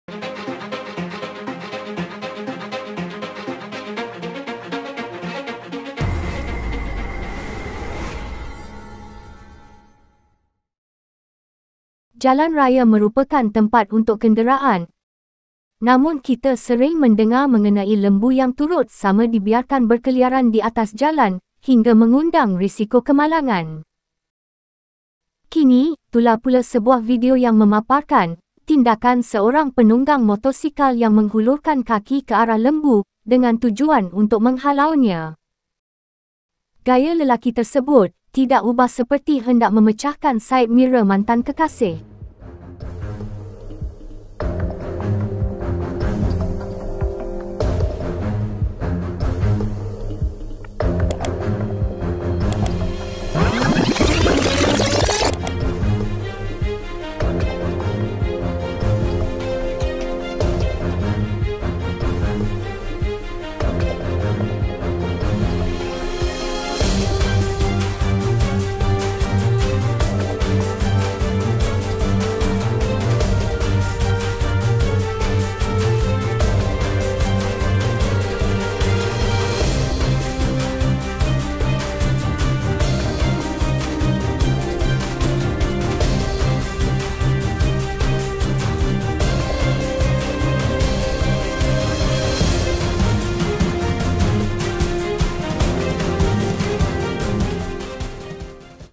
TAK PASAL-PASAL KENA TENDANG
TAK PASAL-PASAL KENA TENDANG memaparkan tindakan seorang lelaki mengusir lembu yang berkeliaran di jalan.